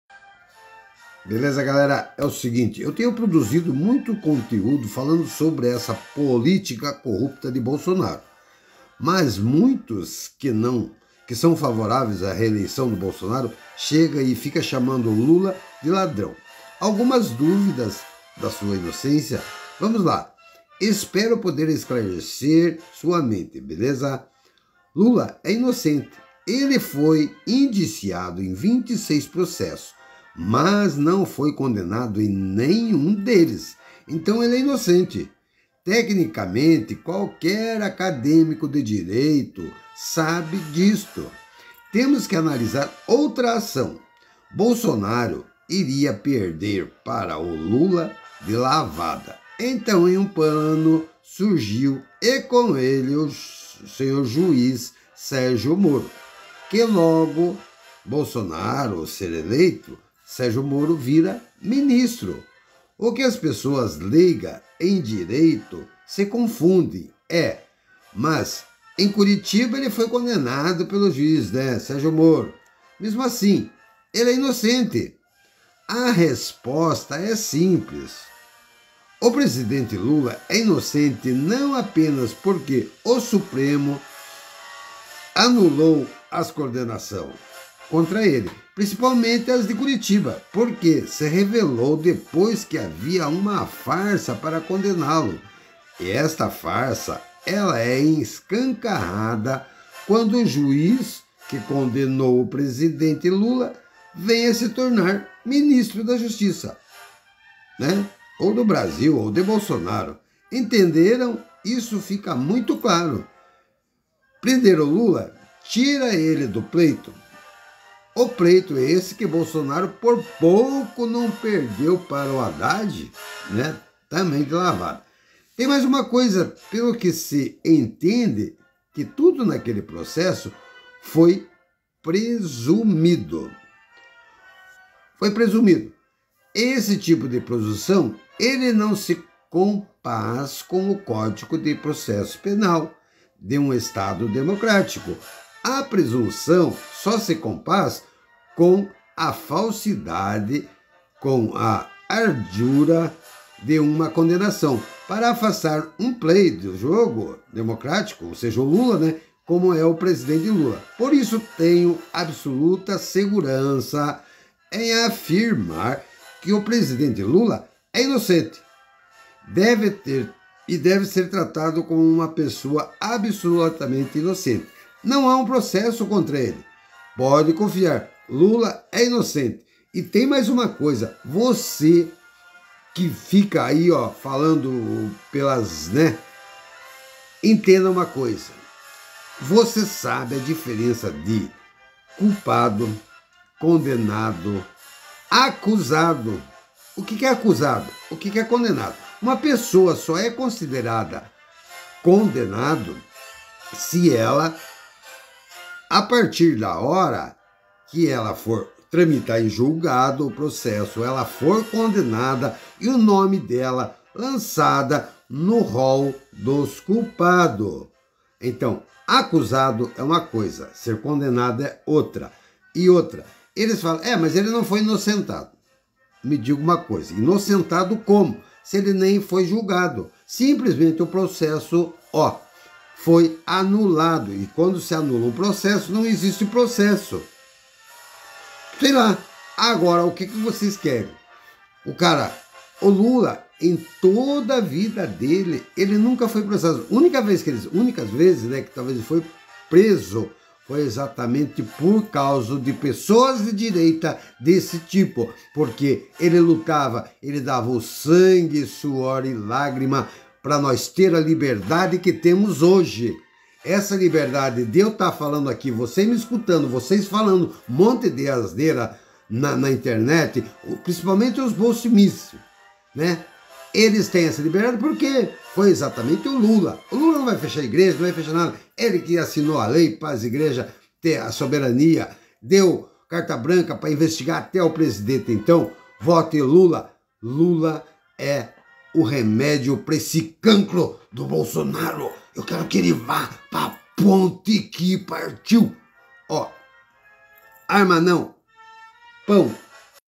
Lula é inocente, saiba Porquê? confira...
Eu estou fazendo alguns vídeos sobre essa anarquia que está no governo bolsonaro, e esclarecendo as acusações que fazem contra o presidente Lula, ele é inocente pelo simples fato que o processo foi anulado, isso acaba com qualquer acusação contra ele, por isso não pode se dizer absolvido ,ele nem foi julgado, uma pessoa só pode ser culpado depois de tramitação em julgado ,condenado e lançado seu nome no rolo dos culpado. acredite Luiz Inácio Lula da Silva é INOCENTE.⚖️🛡⚔️